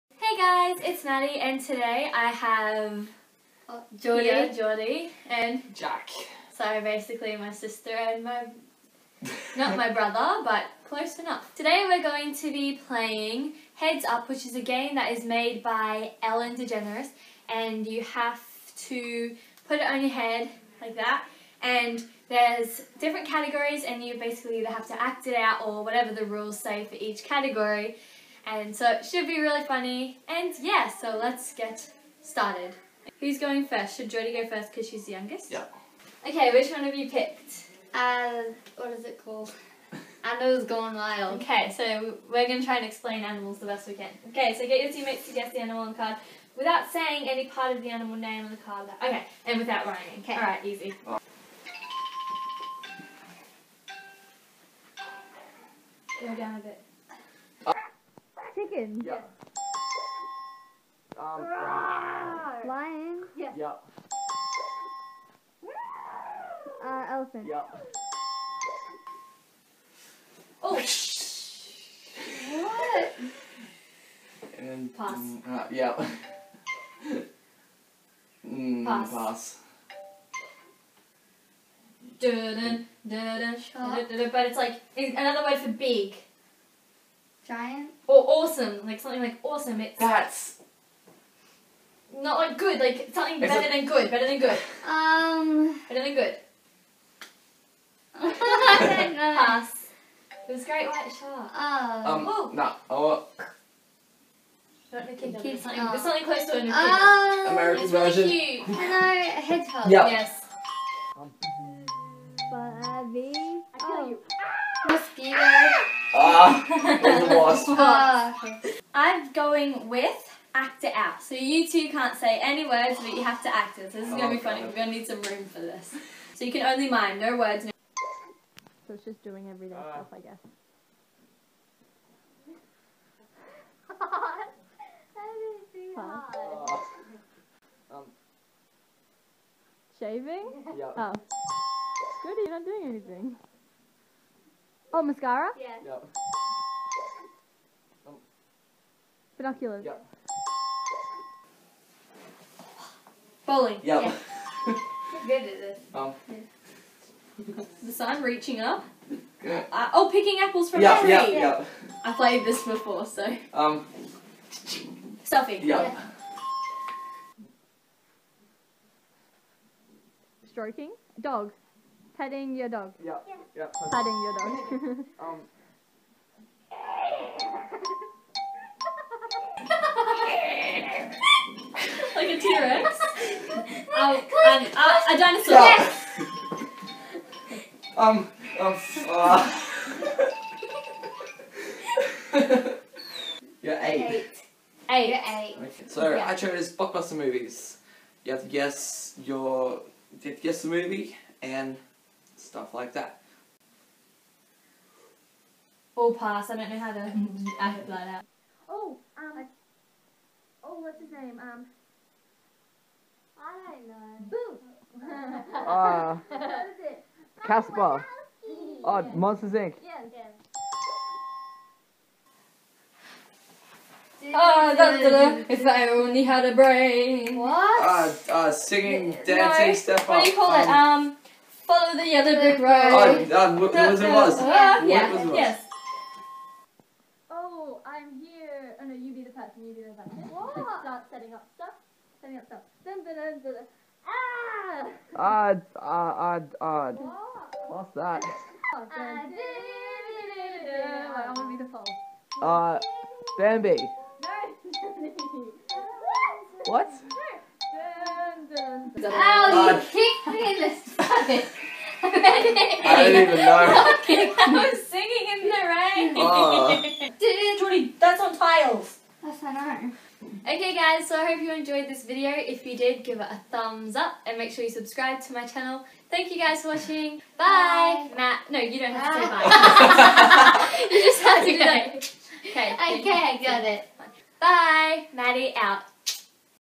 Heads Up II Missy Maddi ft. Jordi and Jack
How many will we guess?

Hello everyone I hope you enjoyed this video :D